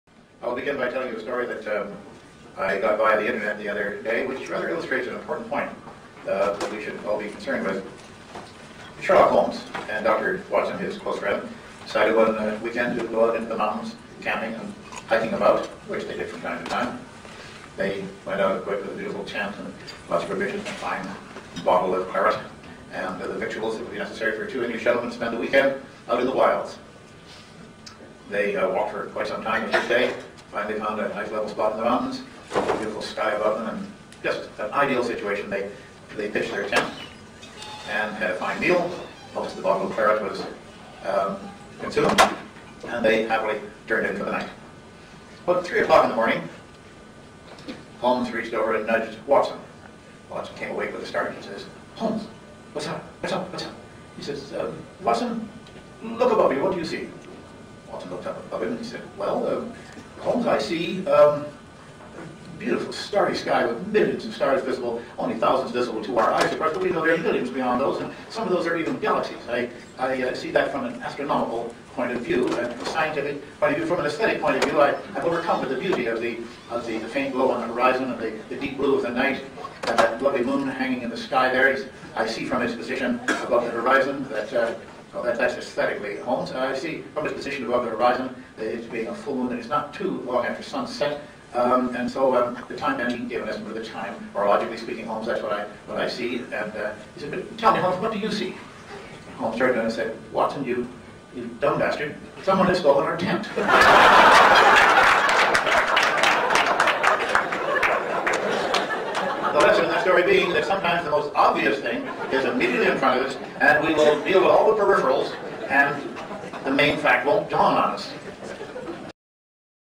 James Randi - Sherlock Holmes' story
Sherlock Holmes' story, part of the video
'' James Randi speaks at CULTinfo Conference ''